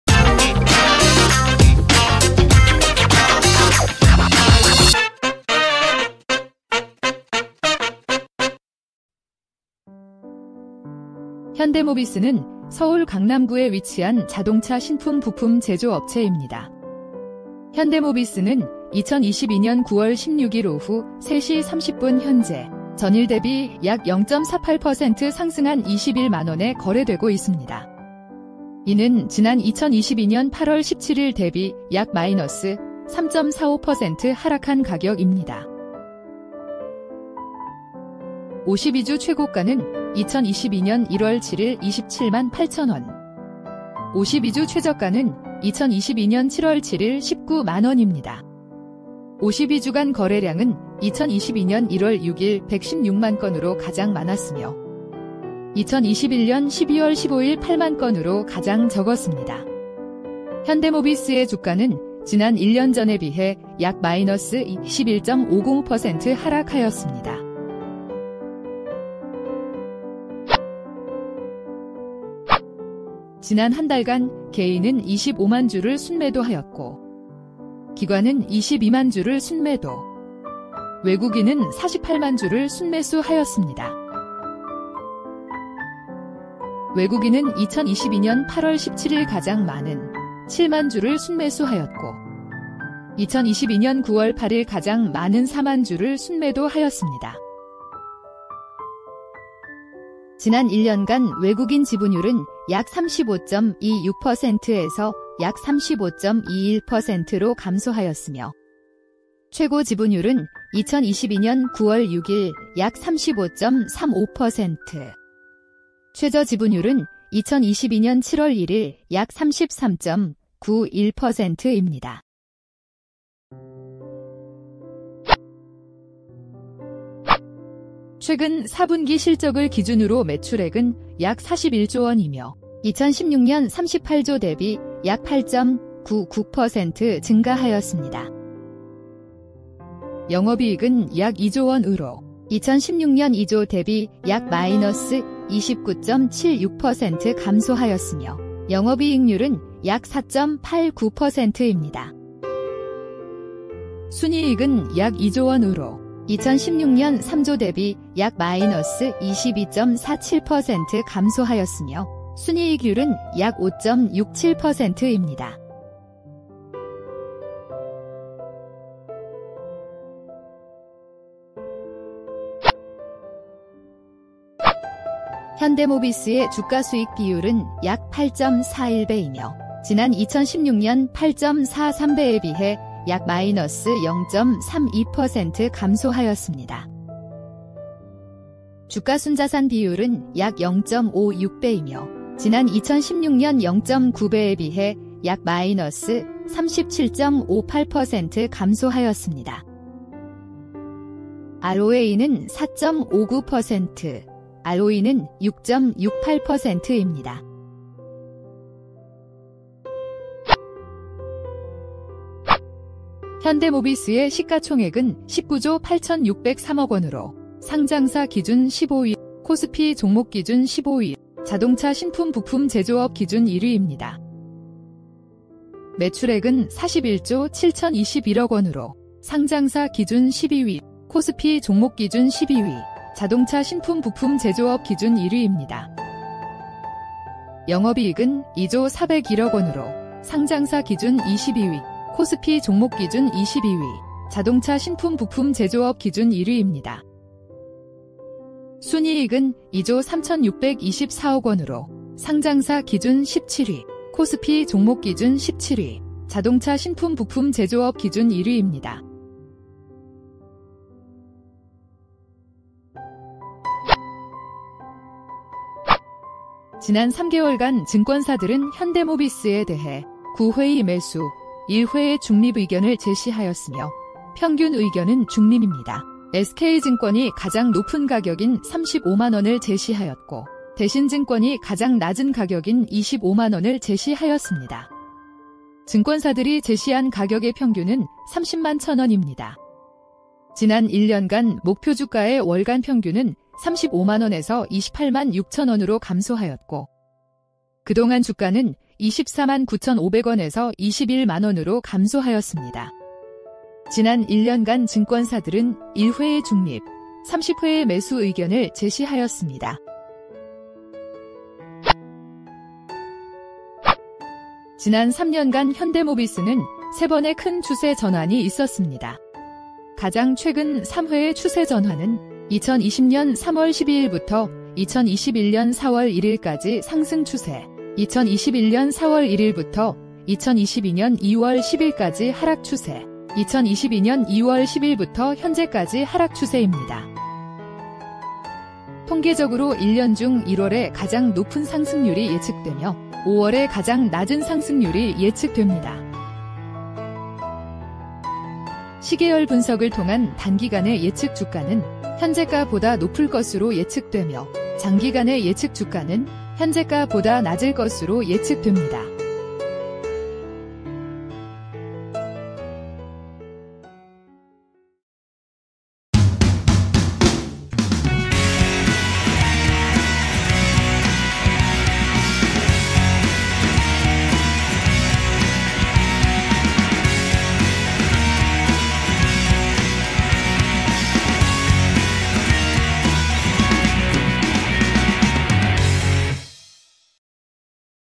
현대모비스주가분석(012330) 2022년 09월 16일기준
현대모비스는 서울 강남구에 위치한
자동차 신품 부품 제조업업체 입니다.
현대모비스는 2022년 09월 16일 오후 03시 30분 현재